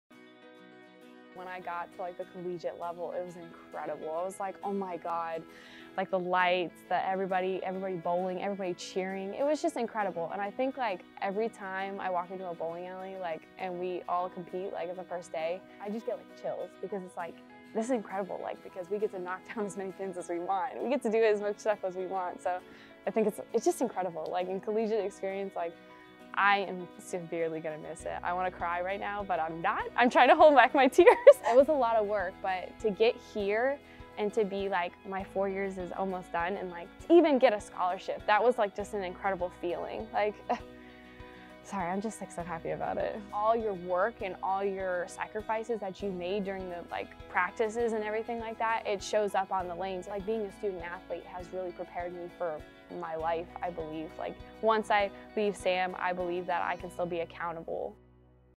Madysen Keller on opportunities college bowling provides
"Being a student-athlete has really prepared me for my life."

Sam Houston State senior Madysen Keller, a kinesiology major, knows her college experience will extend past her four years on campus.